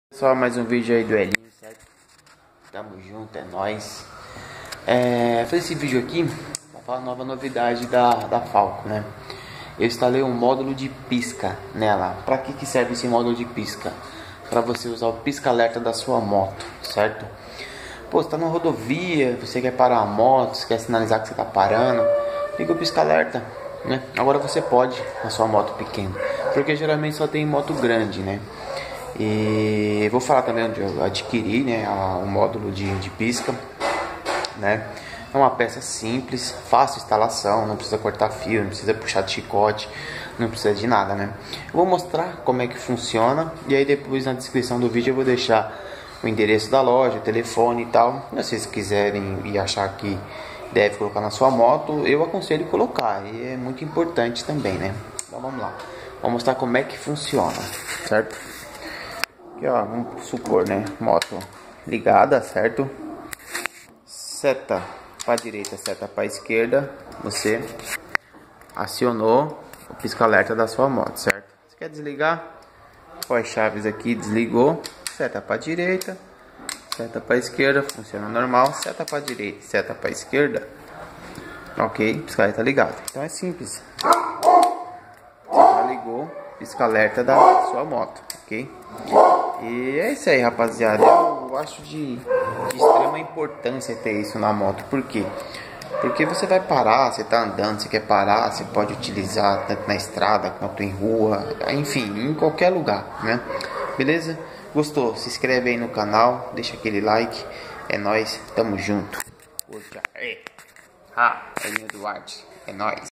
Pisca alerta na sua moto pequena.
Módulo de piscas alerta para sua moto pequena super indico. Abraço do Helinho tmjs.

Estrada do Rufino n° 350, serraria Diadema. Telefone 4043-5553 fala com Fernando.